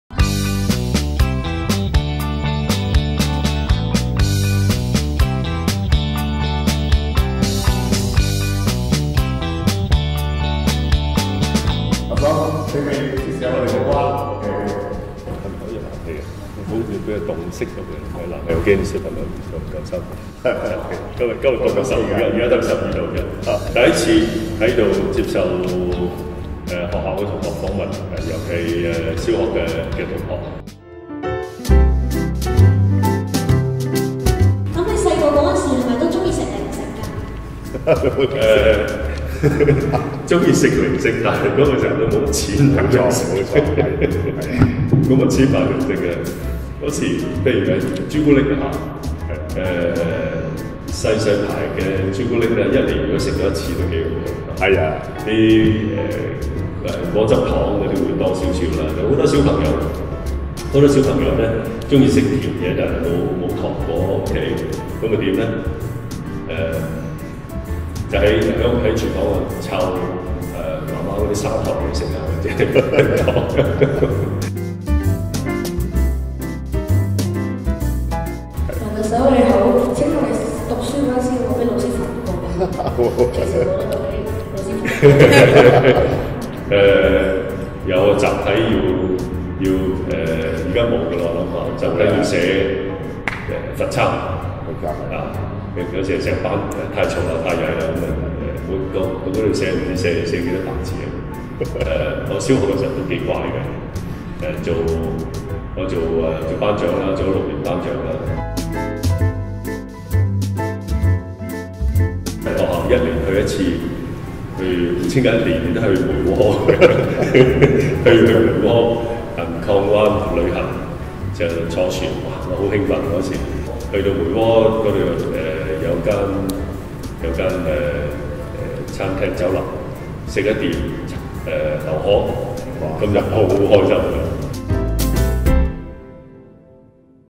【短片】【「暖男」特首與小學生妙問妙答】細個幾乖但畀老師罰？梁振英：細個鐘意吃零食 但好窮無錢買
// 以前小朋友鐘意吃甜嘢但家裡無糖果，咁吃咩？原來會去廚房搵媽媽的砂糖或者冰糖來吃。 //